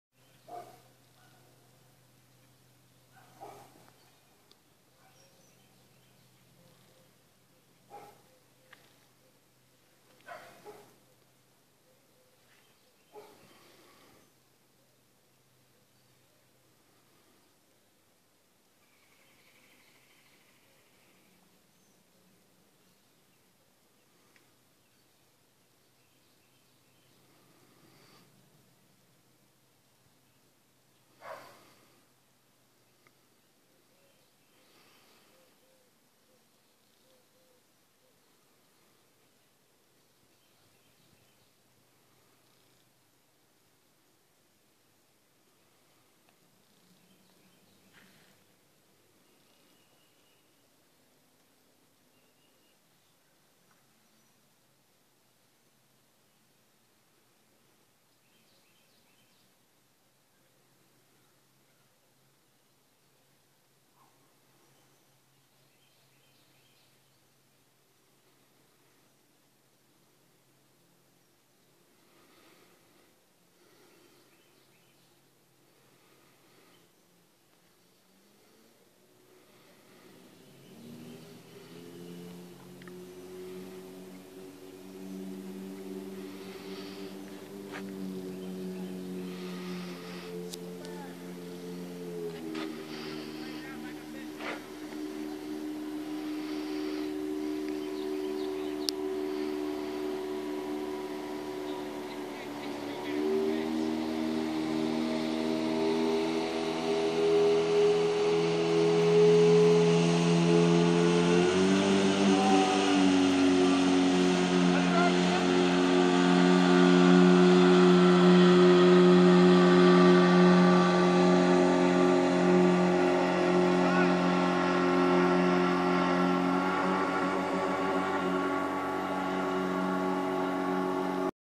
Sailing down the river on a Sunday afternoon.
St George River Astor Florida